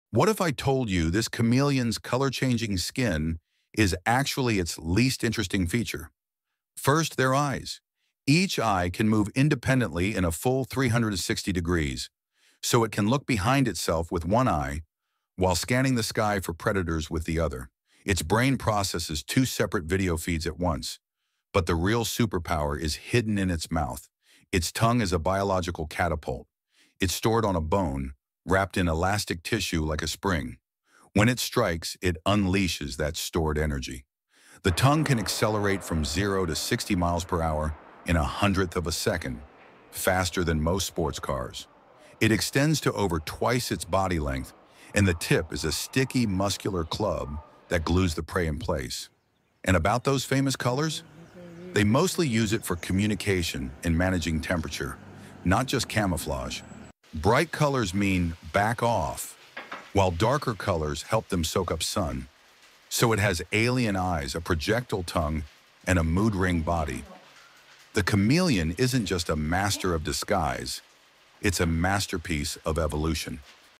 This Animal's Tongue is a Secret Weapon
Think you know chameleons? Their color-changing trick is just the beginning! Discover how their tongues work like powered-up catapults and why their eyes are stranger than any alien.